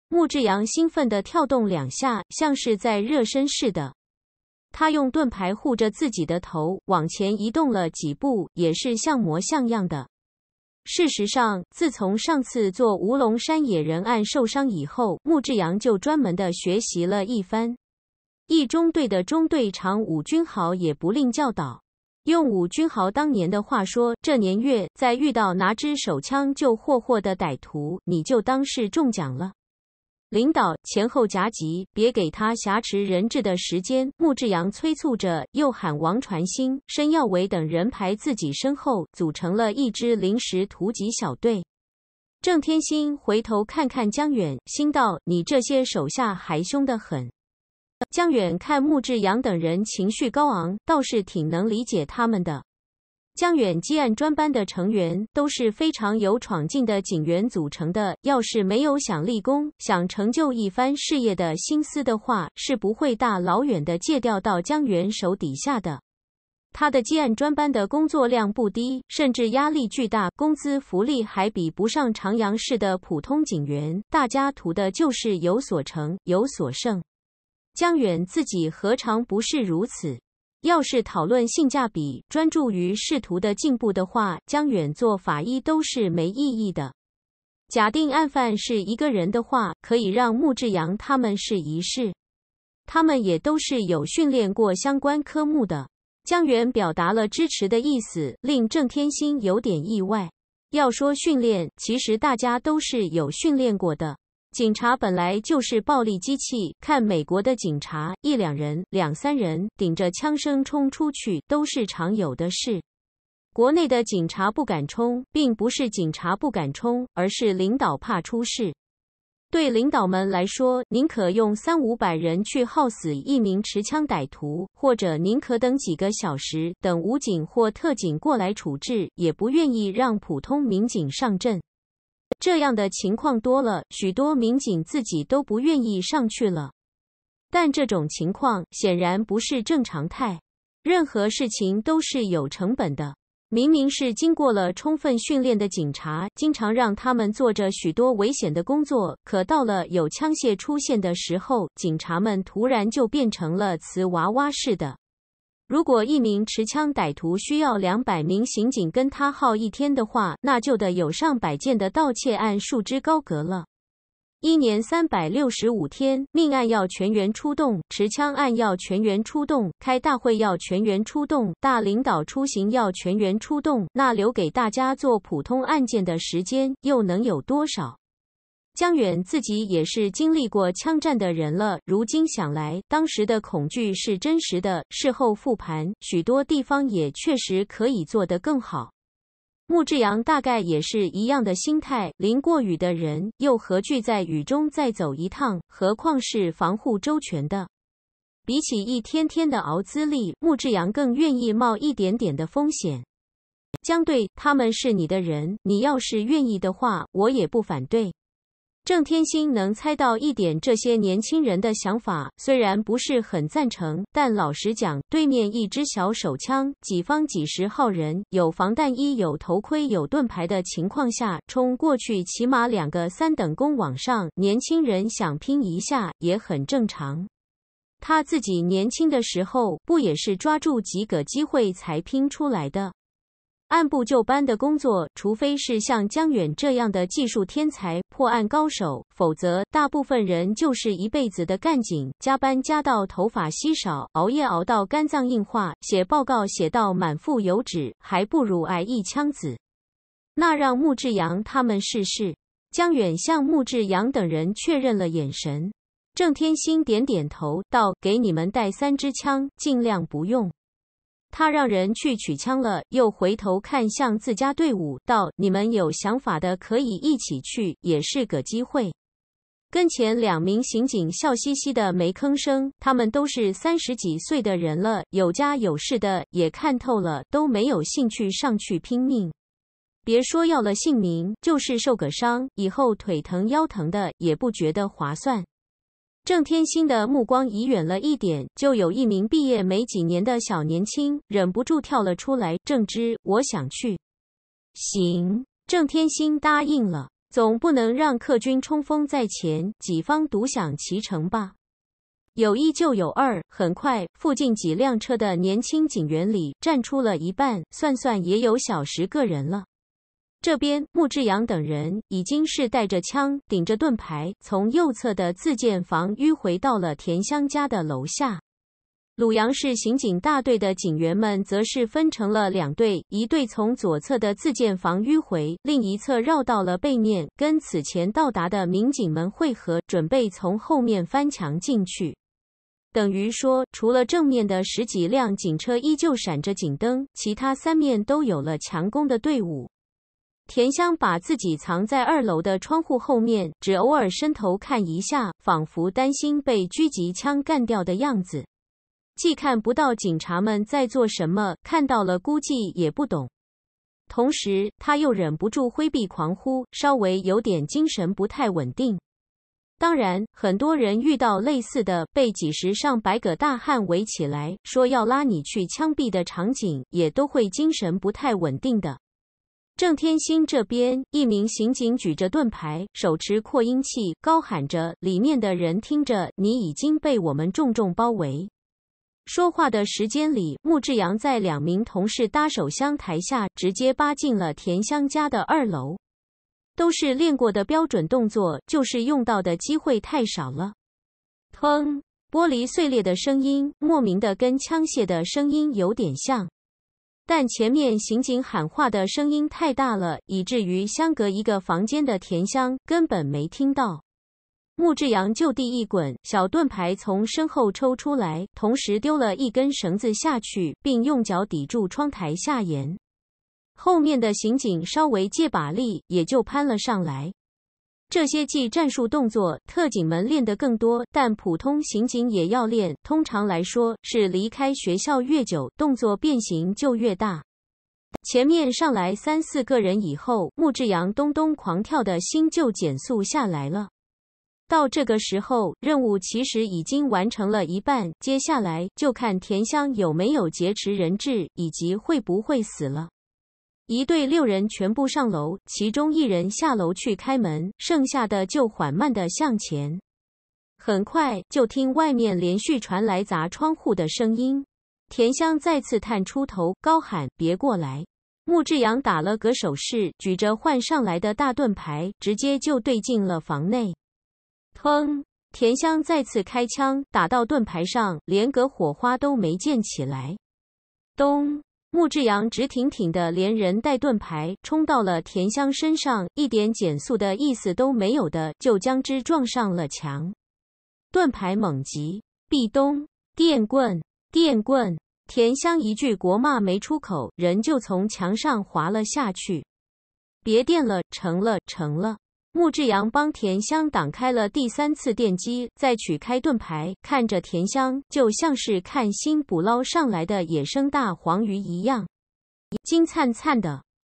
第405章 壁咚《国民法医》
有声小说 都市 商战职场 国民法医 重生过去、畅想未来、梦幻现实，再塑传奇人生！